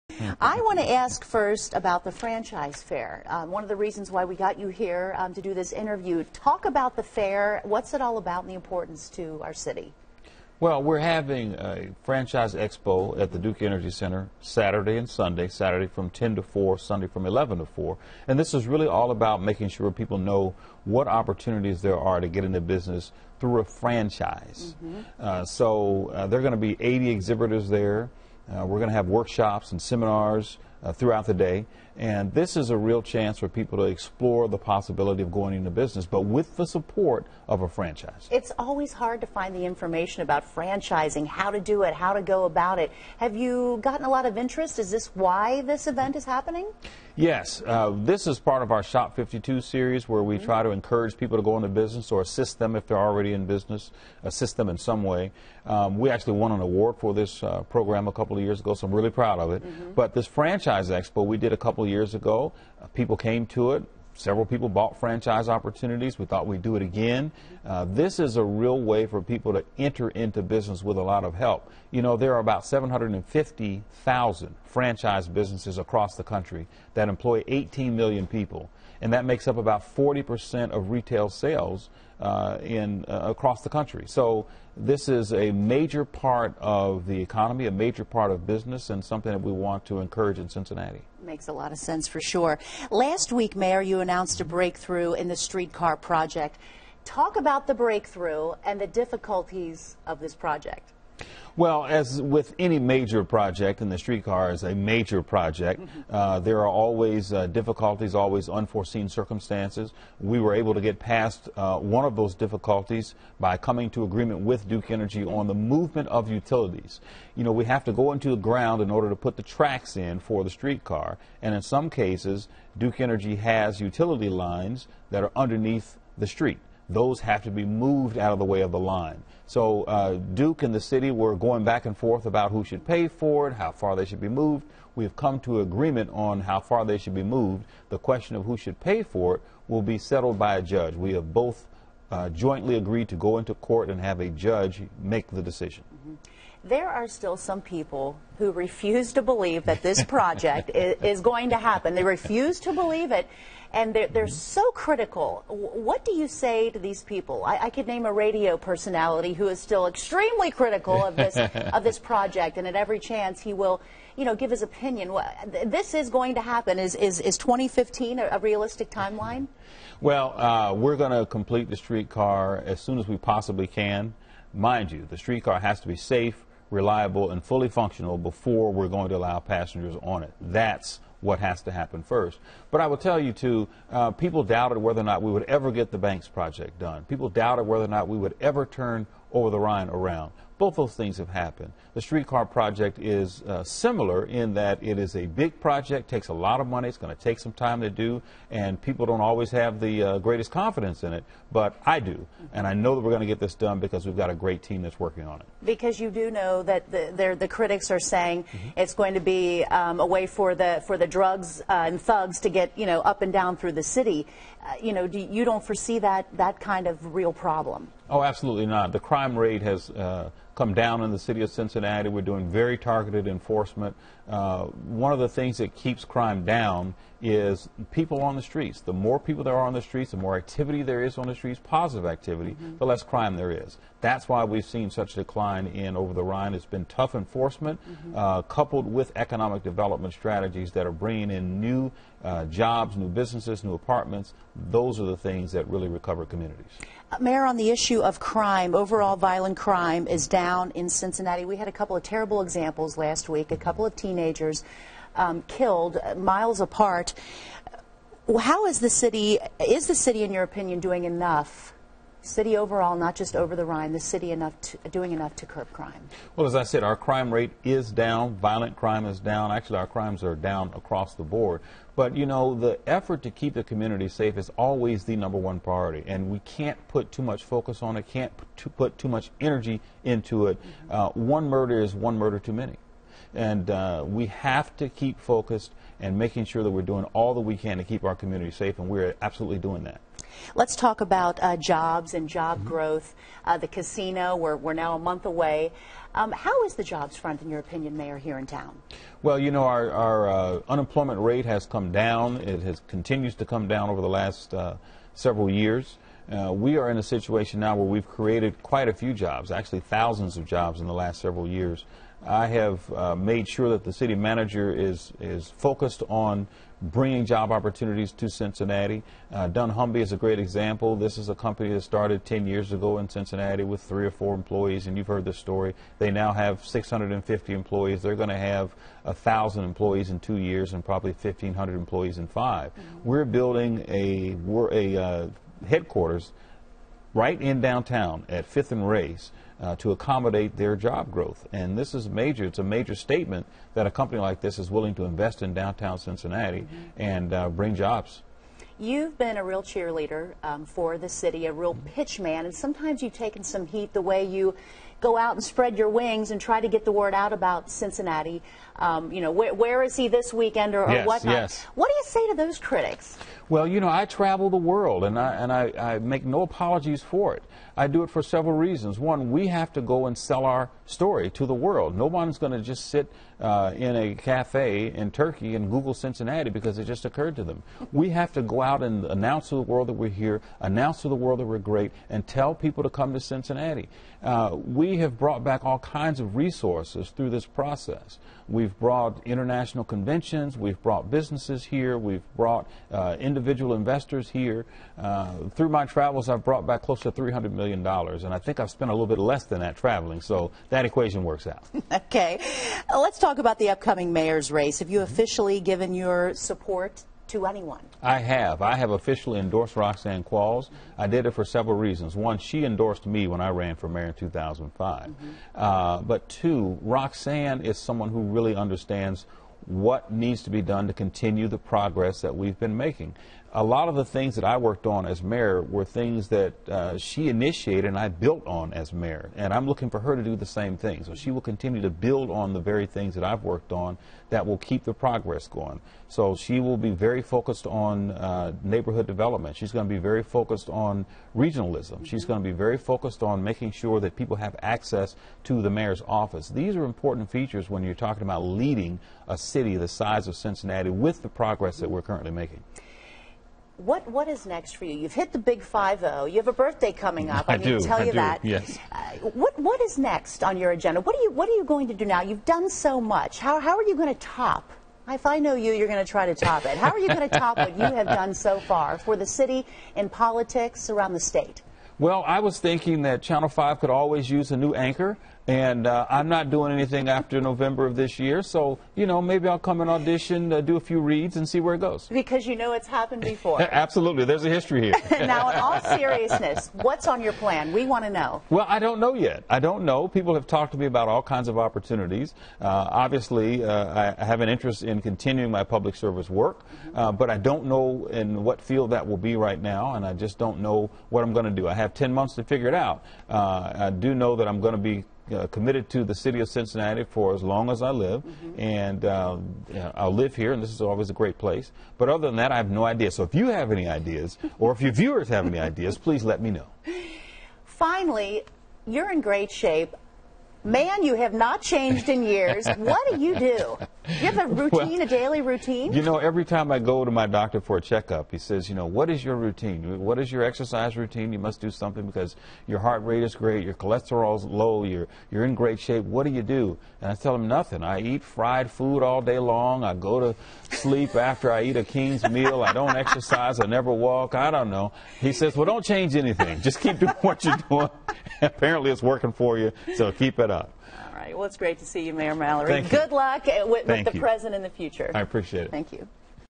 Extended Interview: Catching up with Mayor Mallory
Mayor Mallory talks streetcar, his future and much more